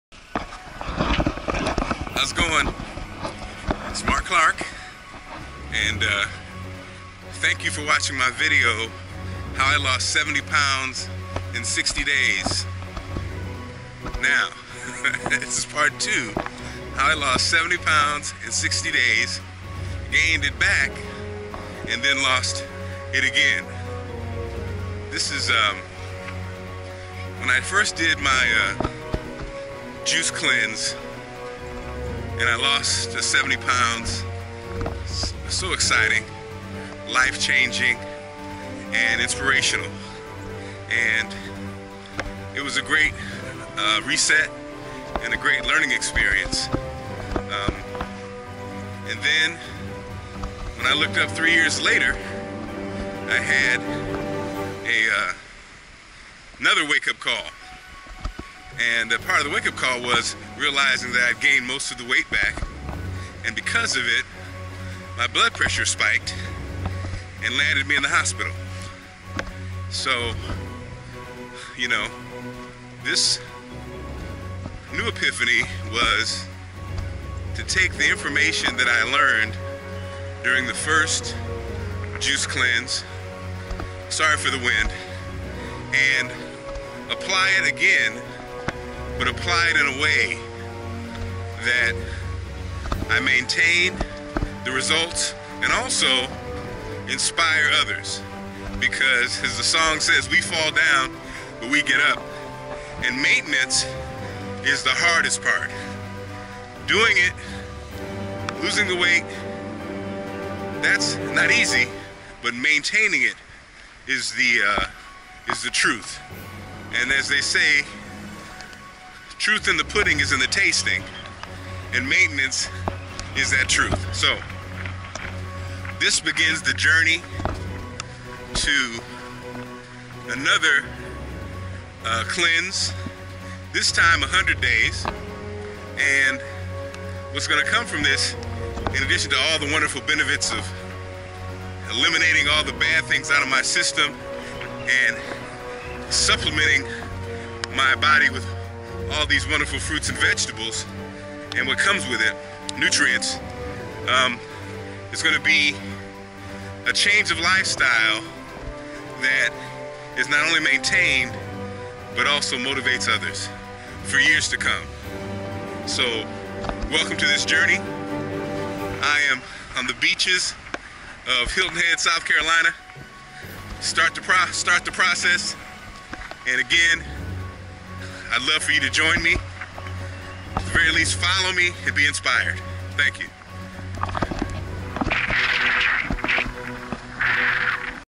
How I lost 70 pounds gained it back and lost it for good
After losing over seventy pounds during a sixty day juice fast Three years later Marc has gained back most of the weight and a recent health scare involving high blood pressure was the wake up call that had him start a one hundred day cleanse with the goal of inspiring others and maintaining the healthy gains.